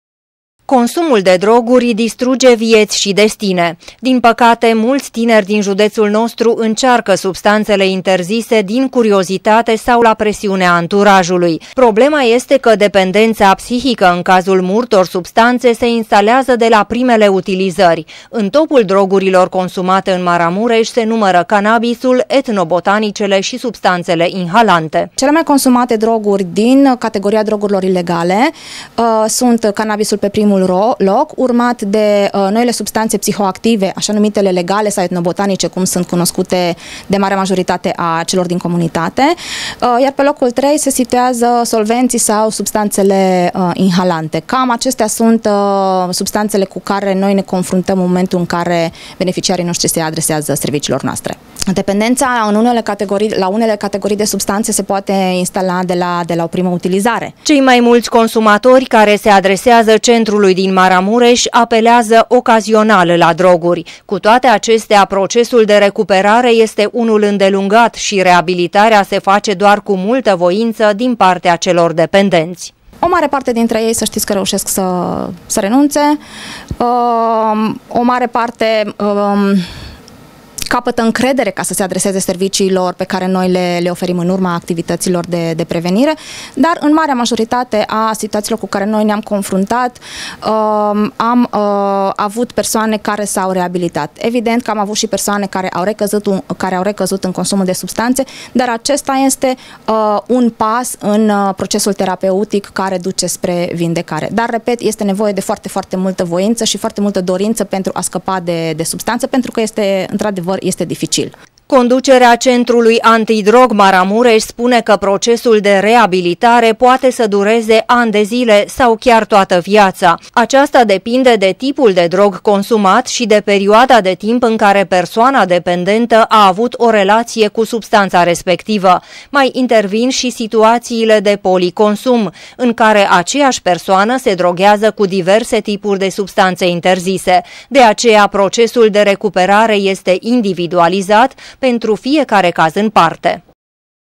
CANNABISUL ȘI ETNOBOTANICELE, CELE MAI CONSUMATE DROGURI
Cele mai consumate droguri în Maramureș sunt canabisul și etnobotanicele, conform utlimelor date statistice. Conducerea Centrului de Prevenire Antidrog Maramureș spune că, în majoritatea cazurilor, persoanele dependente care cer ajutor, sunt consumatori ocazionali. Chiar și așa, acestora le este greu să renunțe la acest obicei.